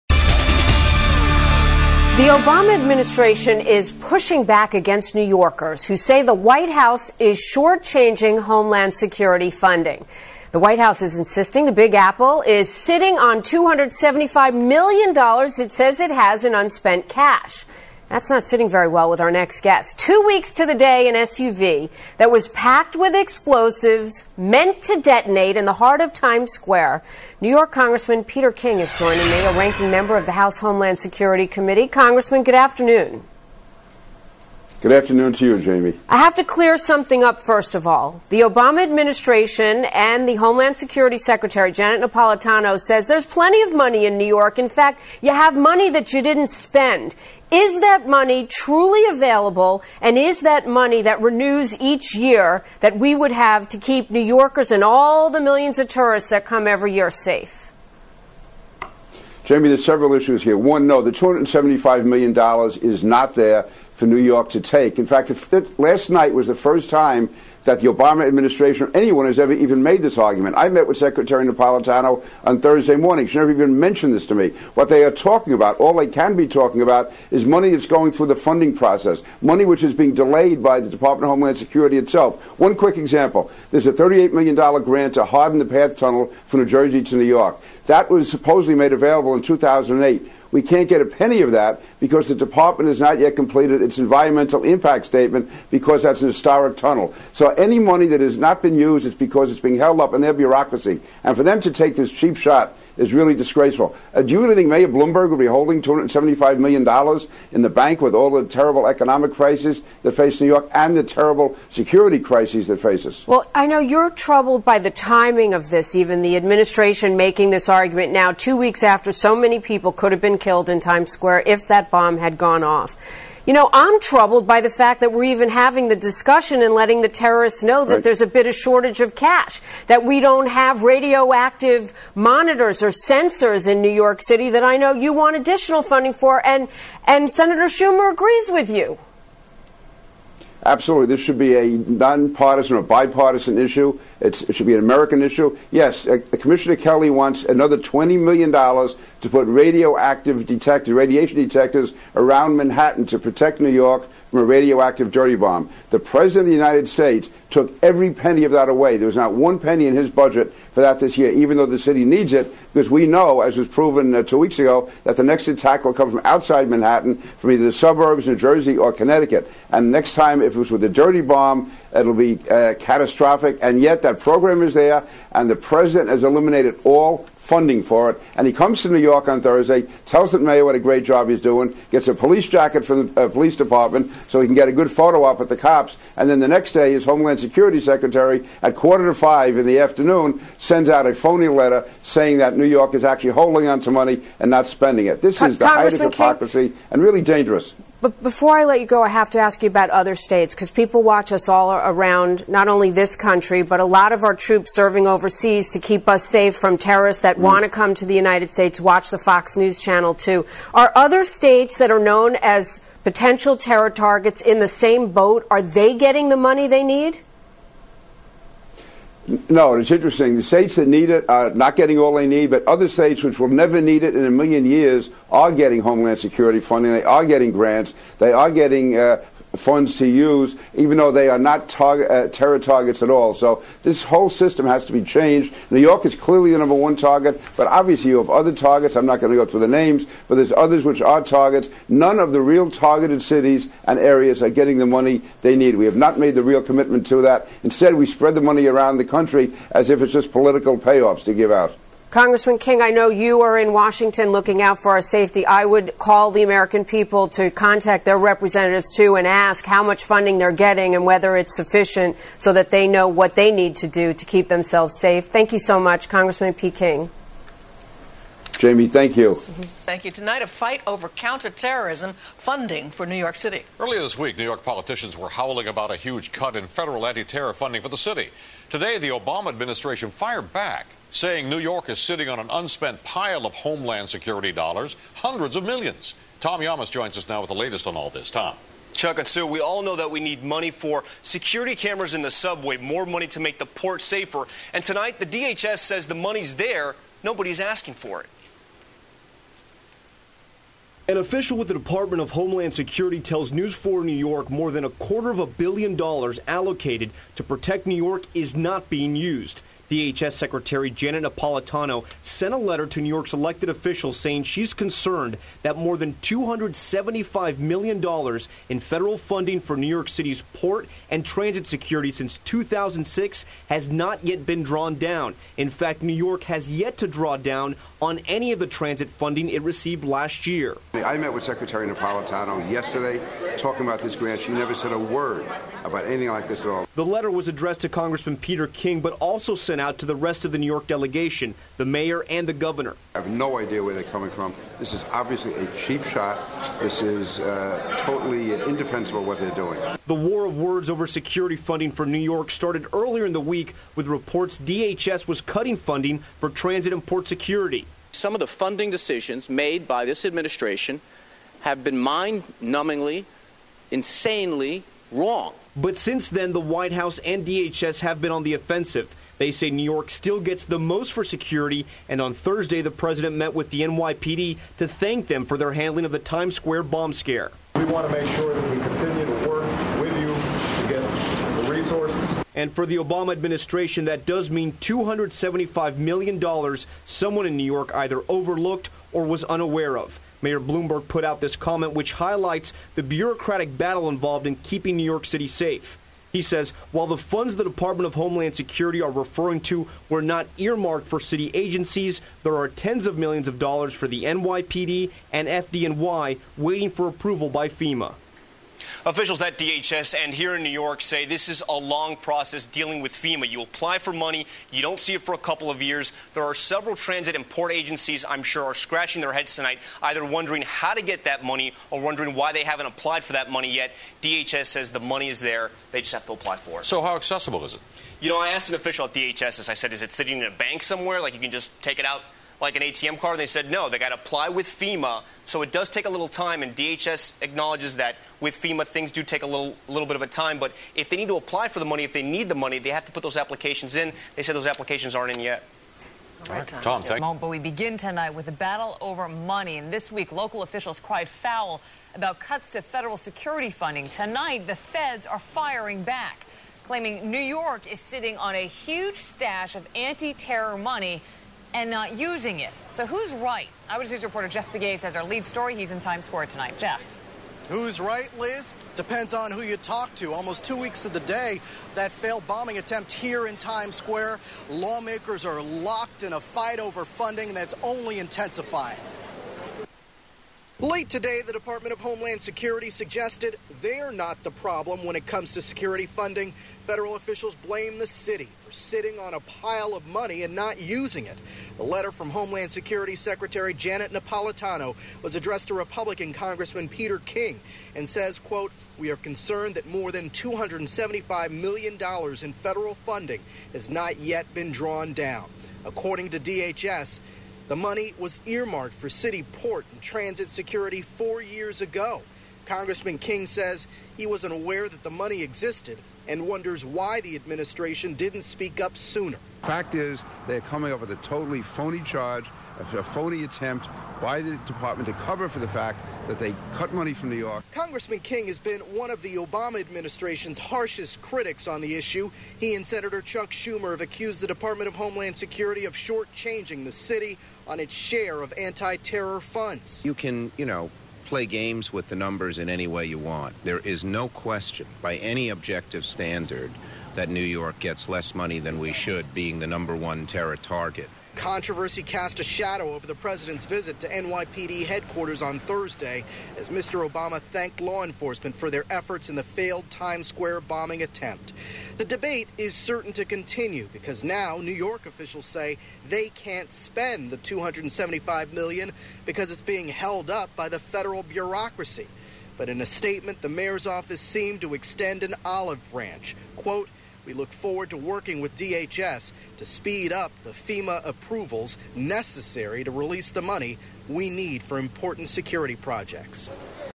Rep. King Responds to Obama Admin. Claims that NYC Sitting on DHS $
Rep. King responds to Obama administration claims that NYC is sitting on homeland security funding.